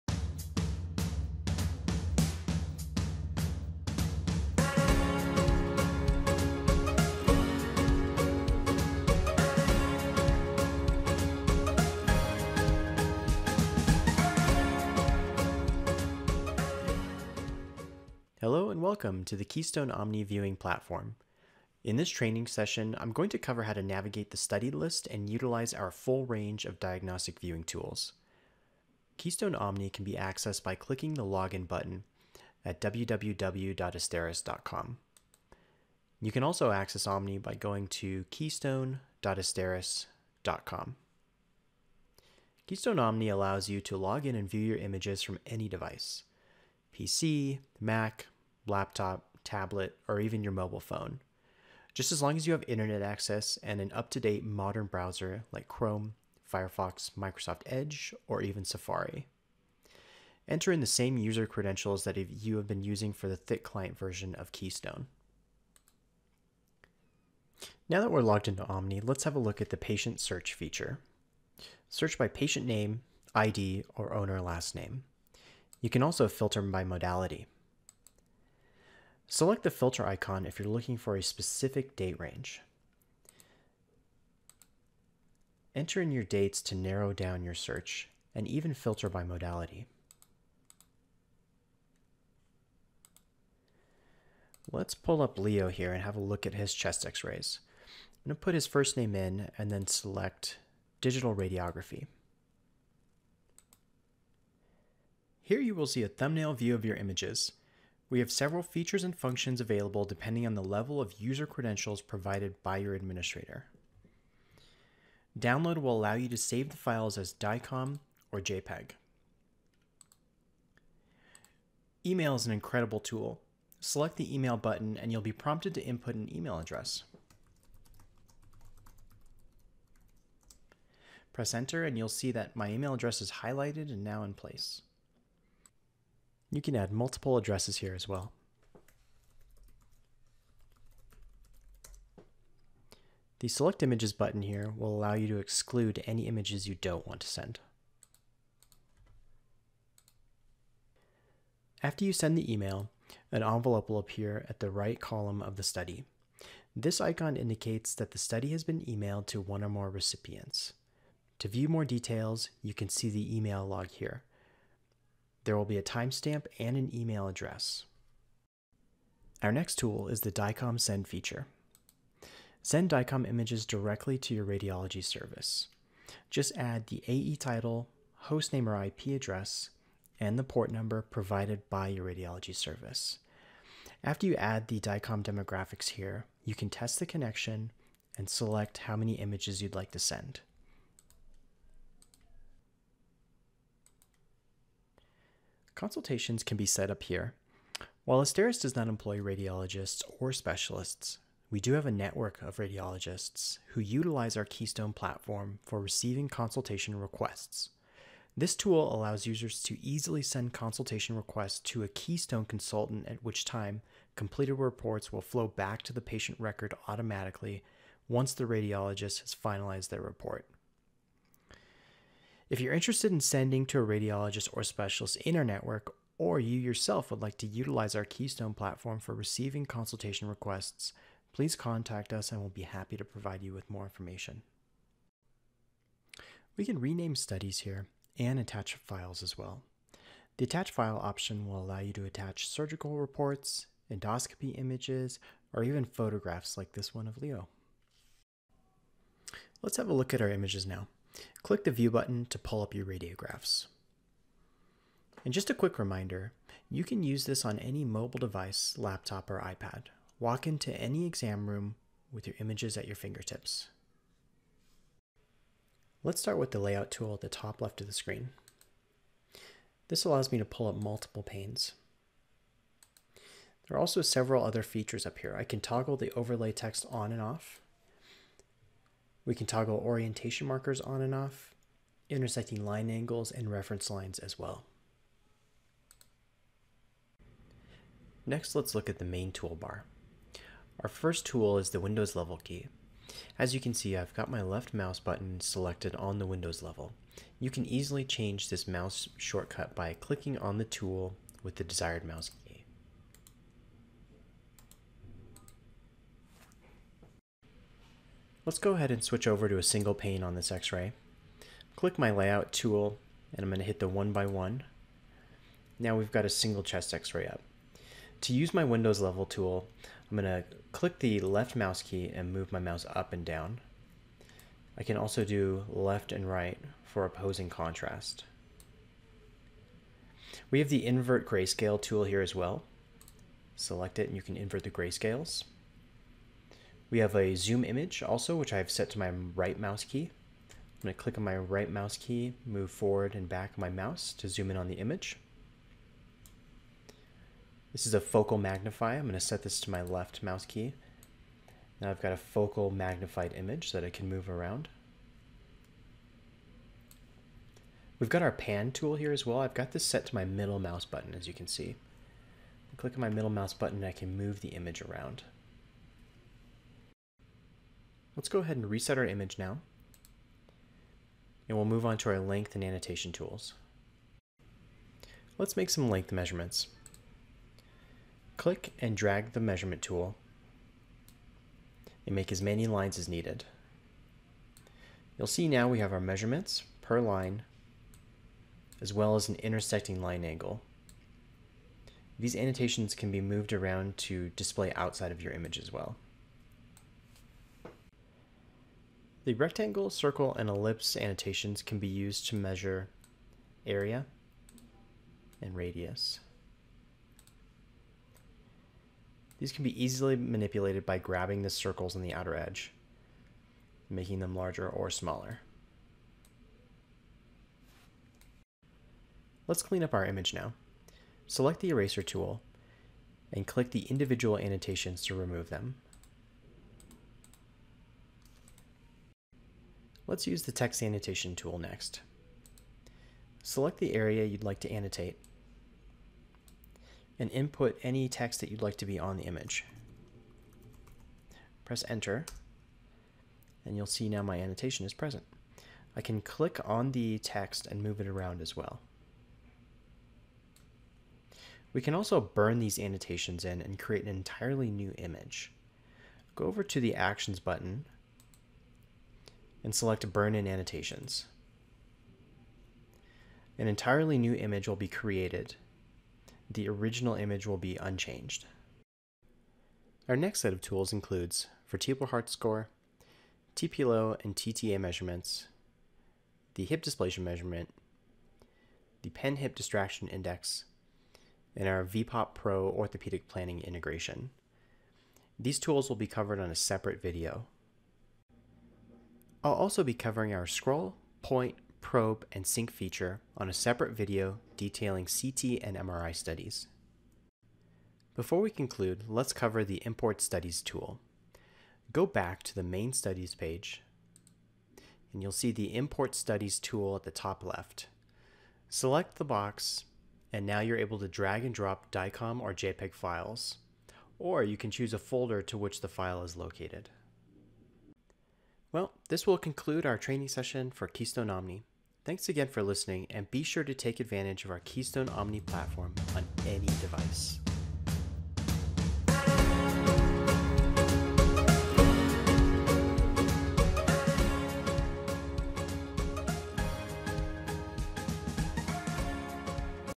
Intro Keystone Omni | BluePearl Telemedicine | Veterinary Image Management Software
Keystone Omni | Browser based viewing of your imaging data. Review the features and functions of the Keystone Omni platform.

Chapters
00:00 Introduction
1:06 Study list
1:23 Study list filters
1:44 Drop down study options -Download / E-mail / DICOM send / Rename / Attach File
2:49 How to DICOM send
4:13 Image viewer
4:30 Multiple panes - overlay text
4:48 Set up mouse buttons
5:15 Windows level feature
5:32 Invert gray scales
5:45 Focal magnify and Pan tool
6:10 Length annotation and area / radius annotation tools
7:36 Burn in annotations
8:25 Import studies tool

All content on this site (including text, photos, videos, music, and artwork) is copyrighted to ©️ Asteris, Inc.